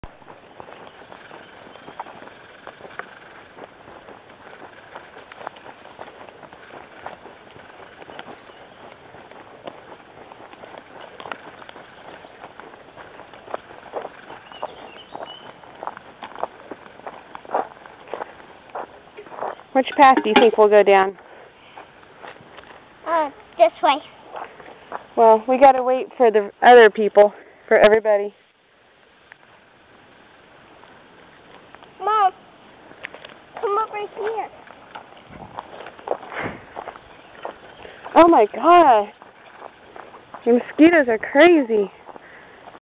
I caught up with KM..
I caught up with Kierstyn on our trail walk. Its the 4th of July, 2010 @ Lakewood Park.